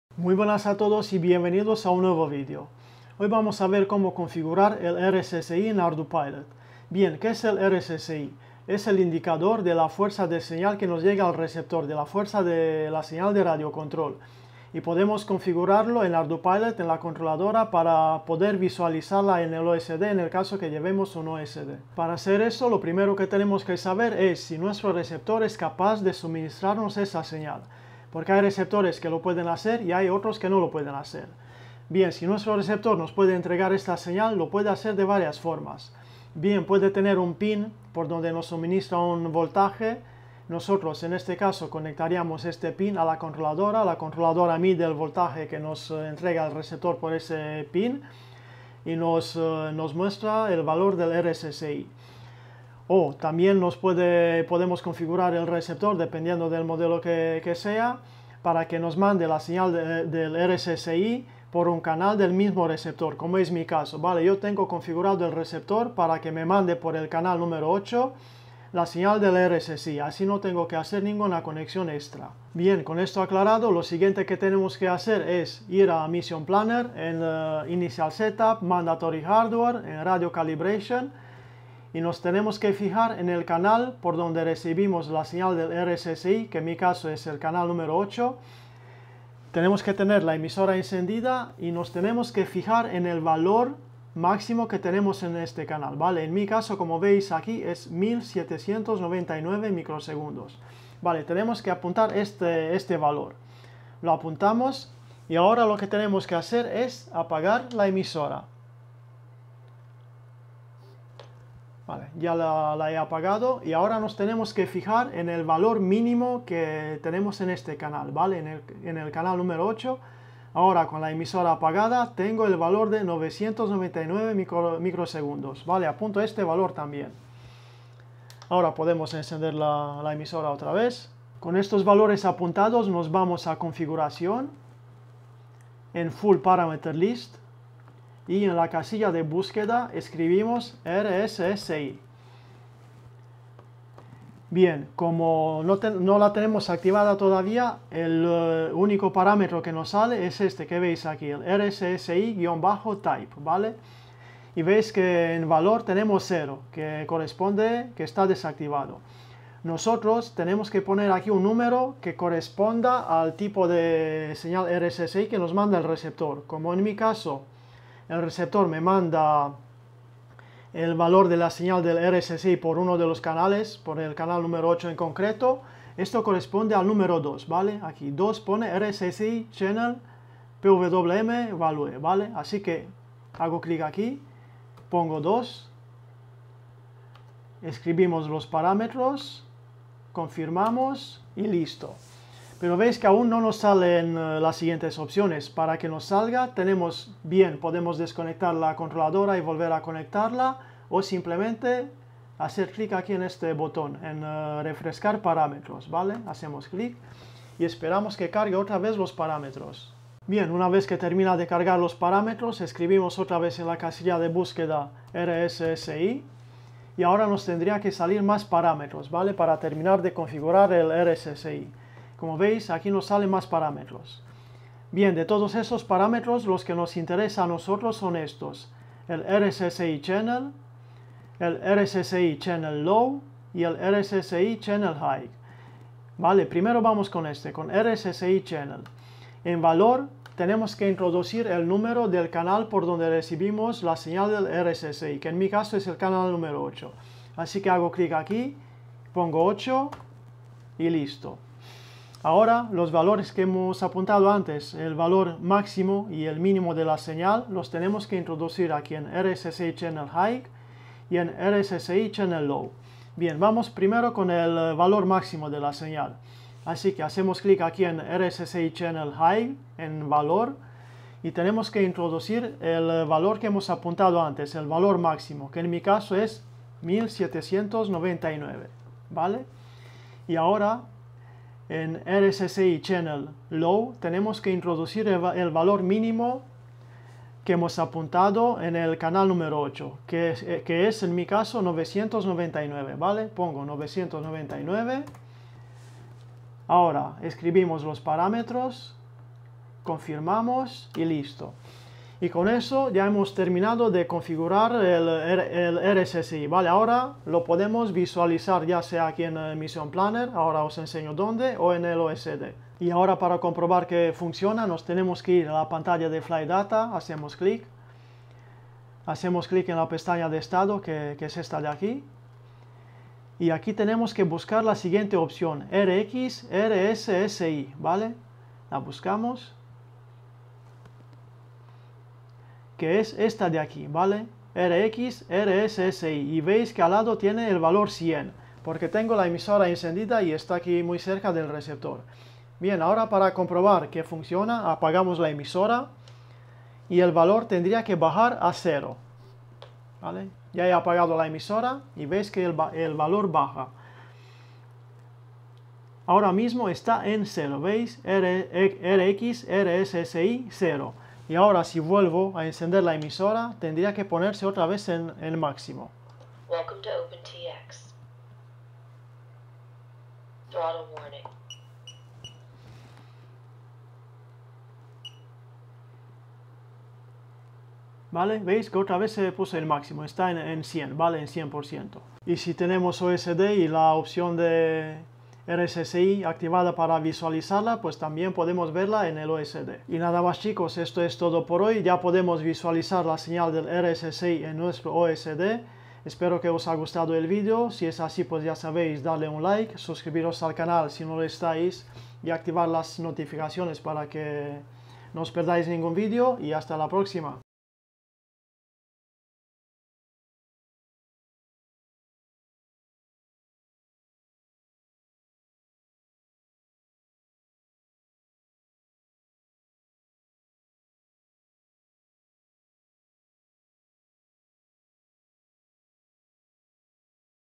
Ardupilot (arduplane) #10 - Configurar el RSSI
En este video os pongo la configuracion del rssi en ardupilot y poder visualizarlo en el osd.

Viseo editado con Filmora 9